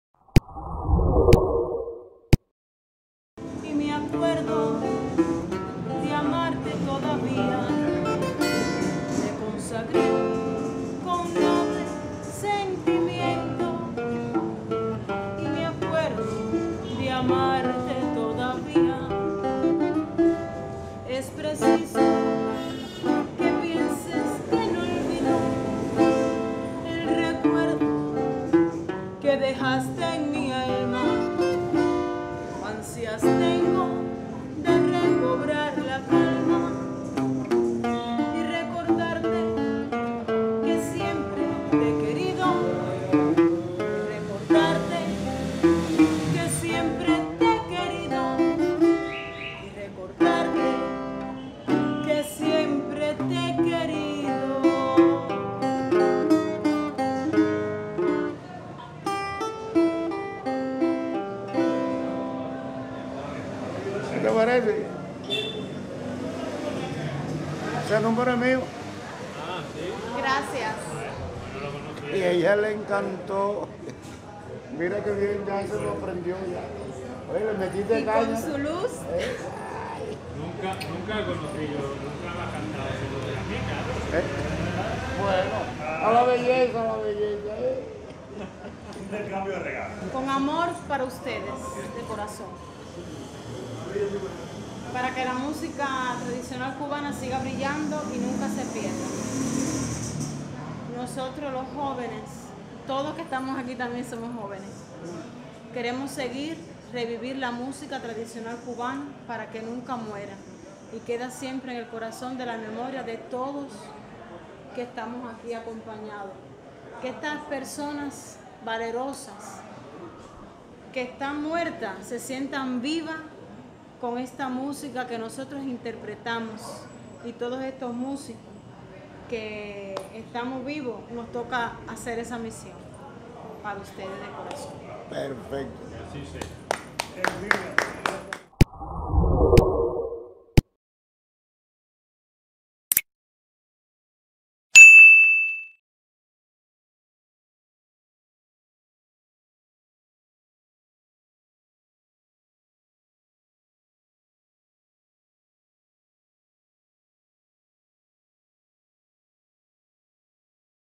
REINA ROSA COBAS CON ALEJANDRO ALMENARES
Alejandro Almenares acompañando un tema de su autoria a Reina Rosa Cobas